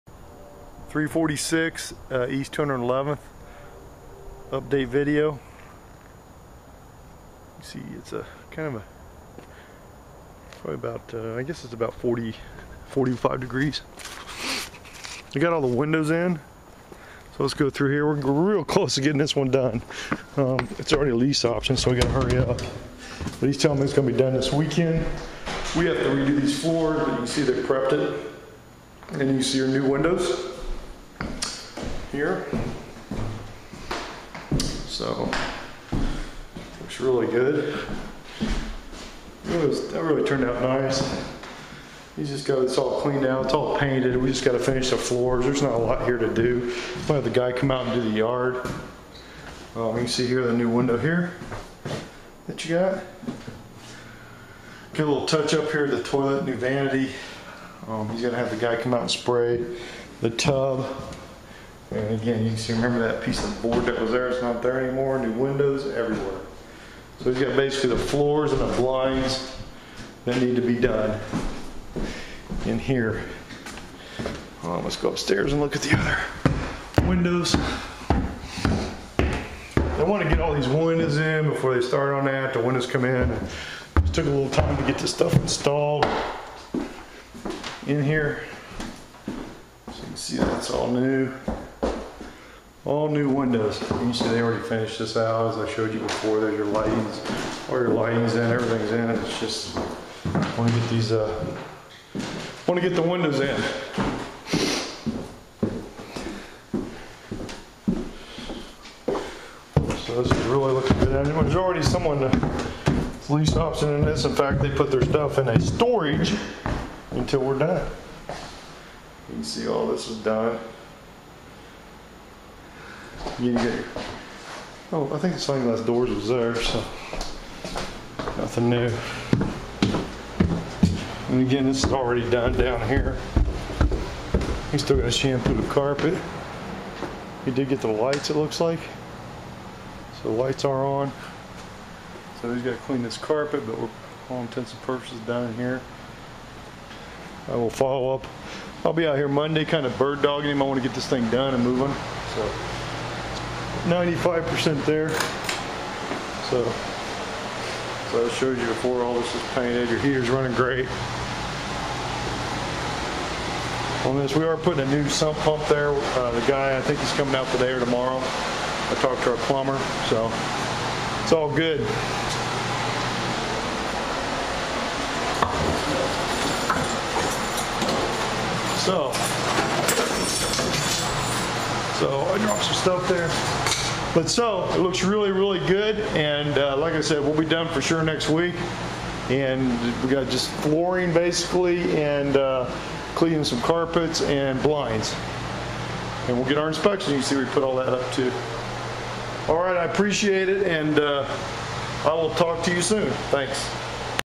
346 e 211 Update5..MP4
Great 3 bedroom 1 bath home with finished basement and brand new windows throughout!